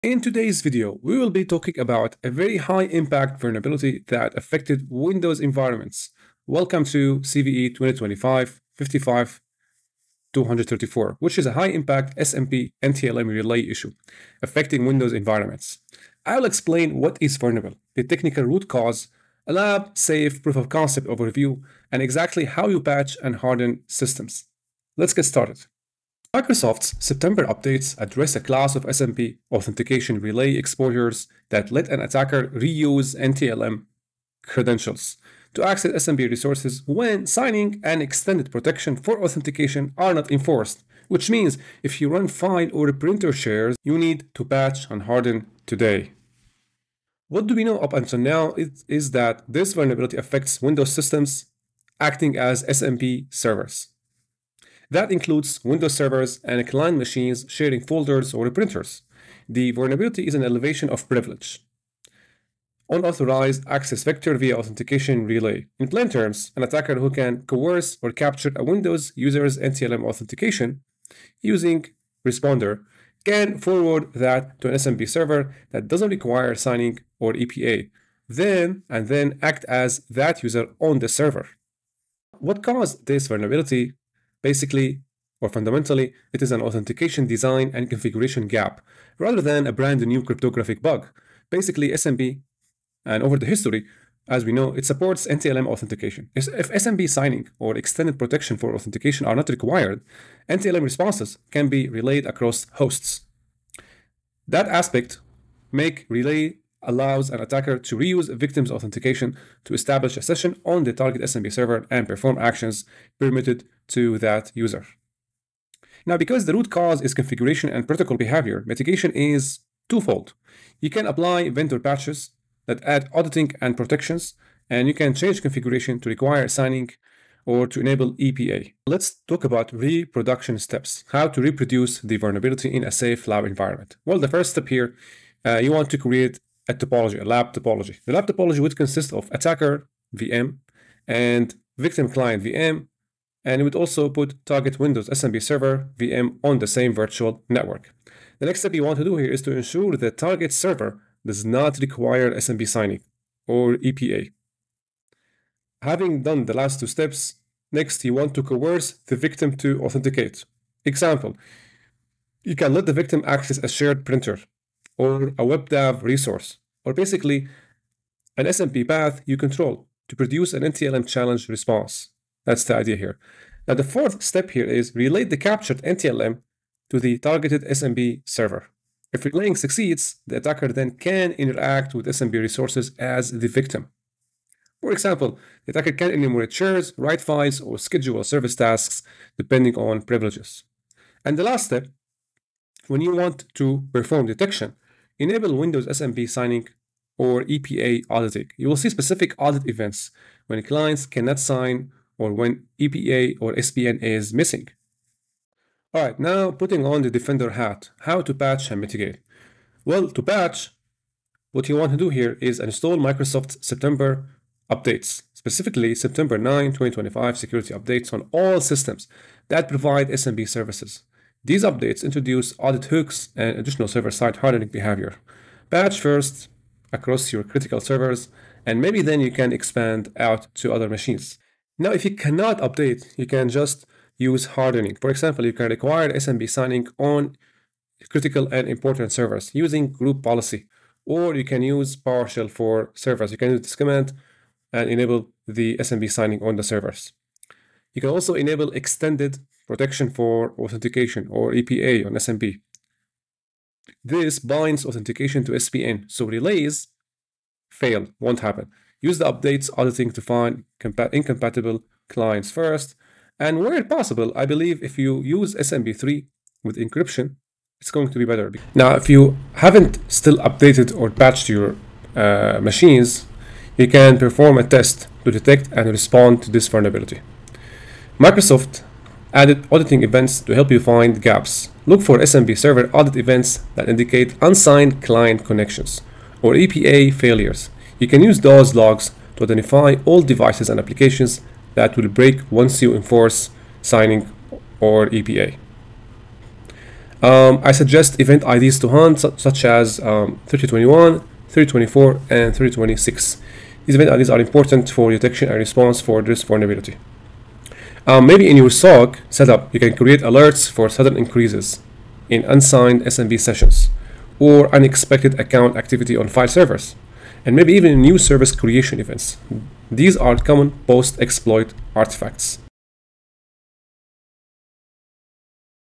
Critical Microsoft SMB Vulnerability | CVE-2025-55234 Explained
👉 Download The Ultimate CVE Timeline (2010–2026) Cheat Sheet now
Deep dive into CVE-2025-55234 — what Windows systems it affects, the technical root cause behind SMB/NTLM relay exposure, a lab-safe PoC overview, and a step-by-step mitigation playbook (patches, GPOs, PowerShell, and detection).